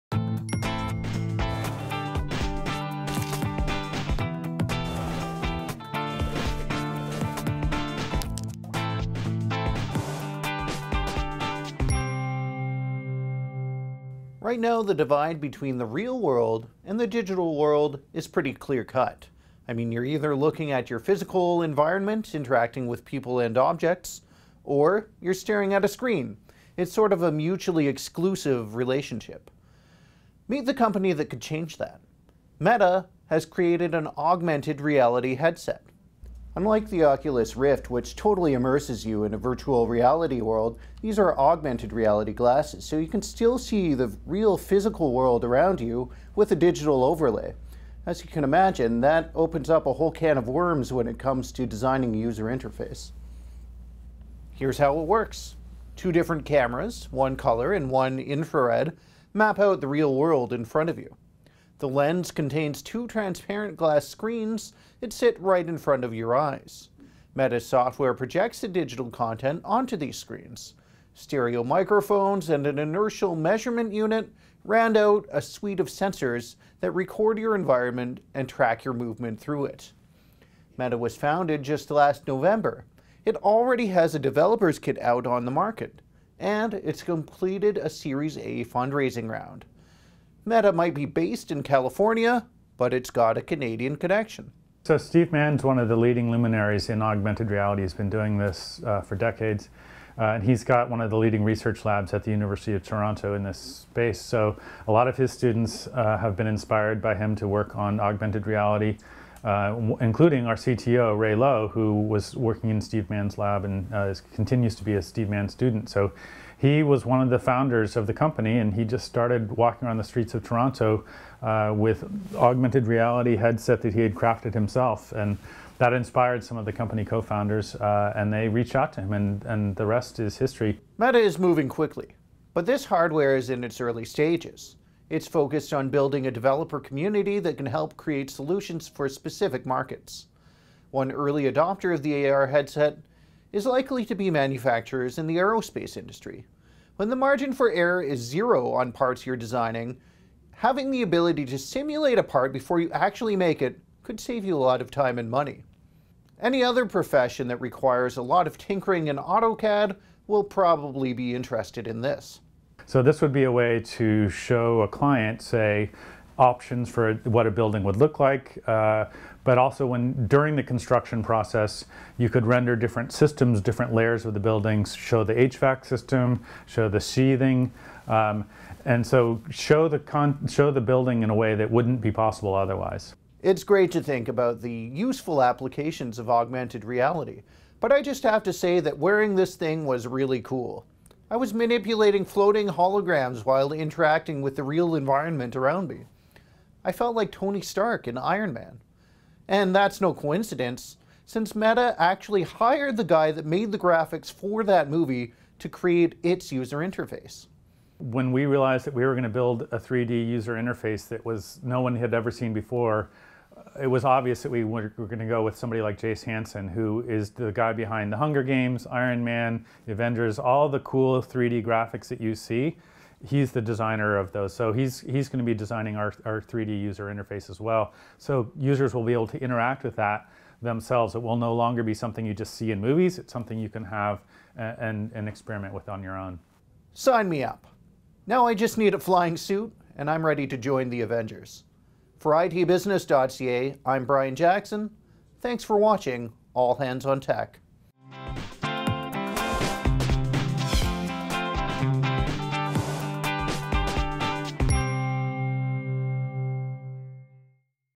All Hands on Tech: Meta augmented reality glasses
Unlike virtual reality headsets that totally immerse you in a digital world, augmented reality means you're overlaying a digital experience on the real world around you. Meta is the California-based team that is looking to perfect its headset to provide this experience, and it's got a Canadian connection from the University of Toronto. Plus, find out how this company is connected to Ironman.